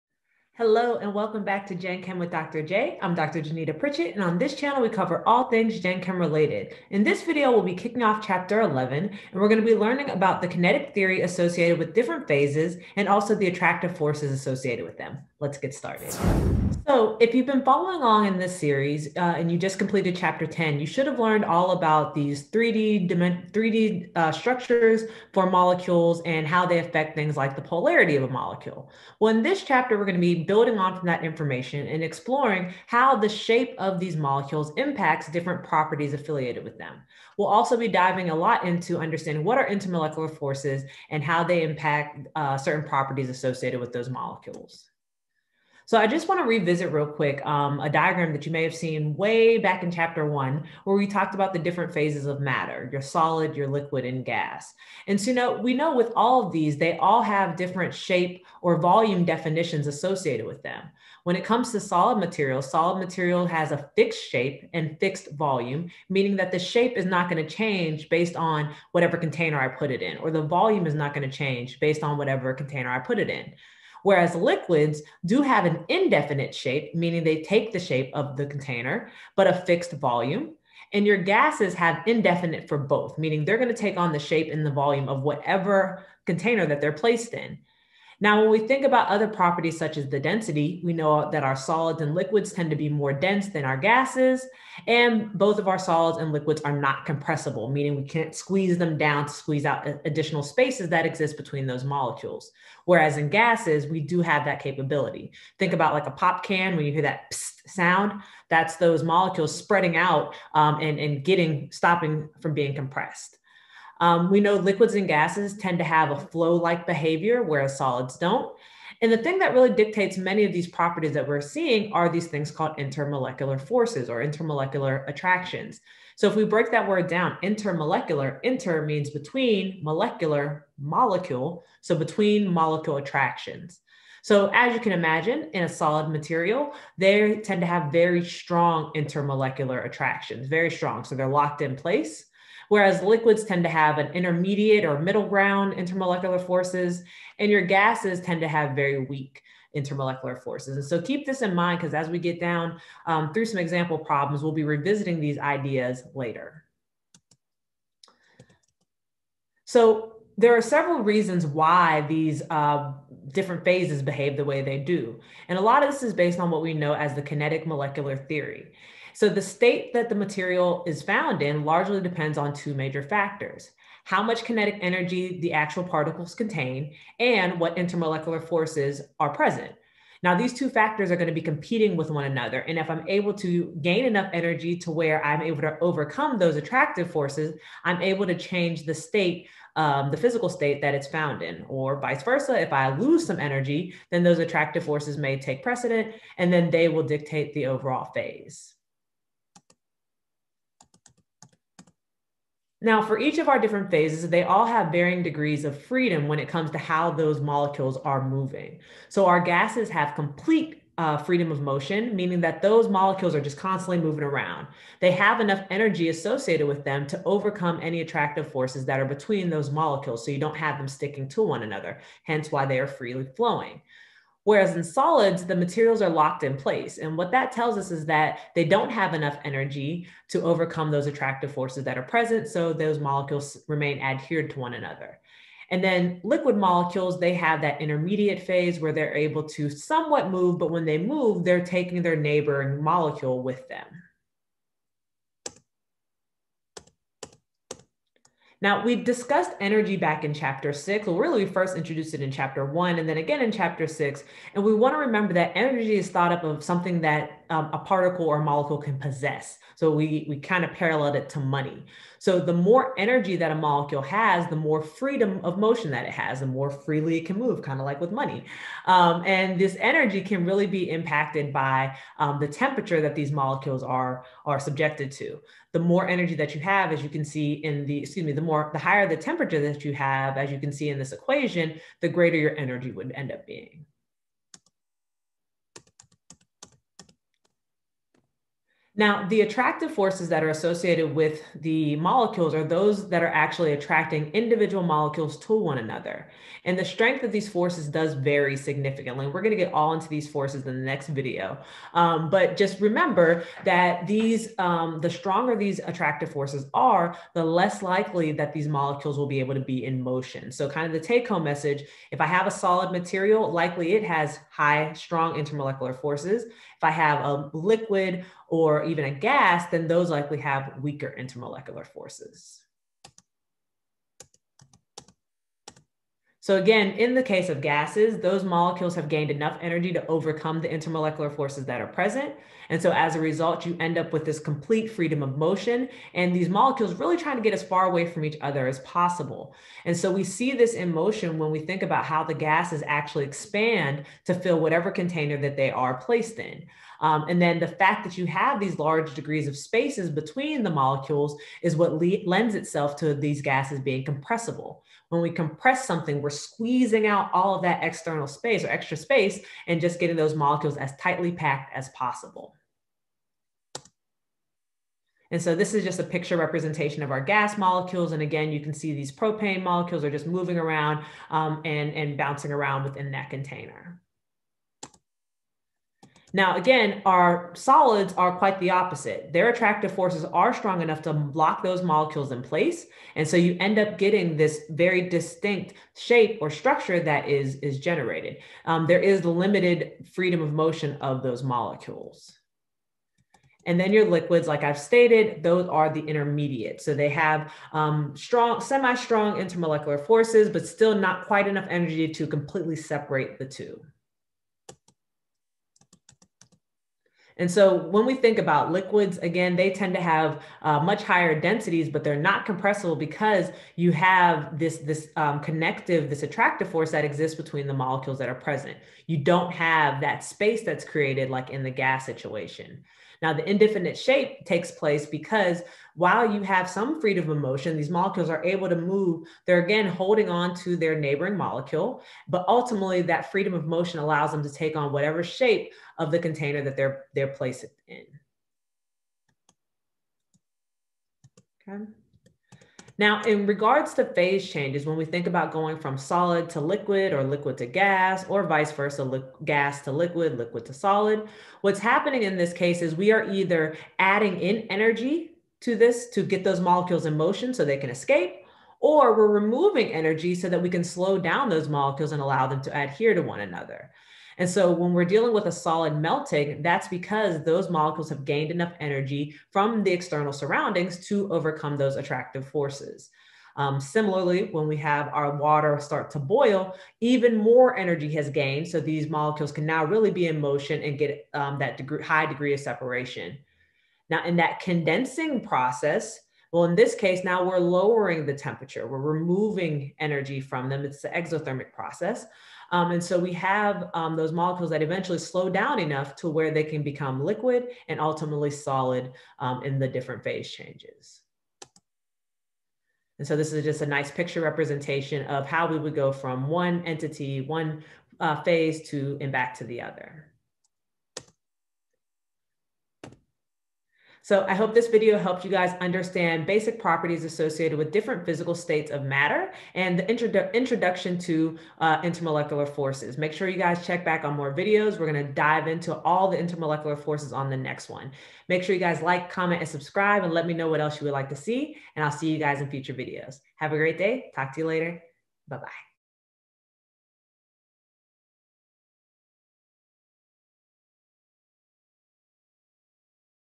Kinetic Theory and Attractive Forces | Gen. Chem With Dr. J
This video covers kinetic theory associated with different physical states and attractive forces.

Slides: Adapted from Pearson Education

LinkedIn: Jeanita Pritchett, Ph. D.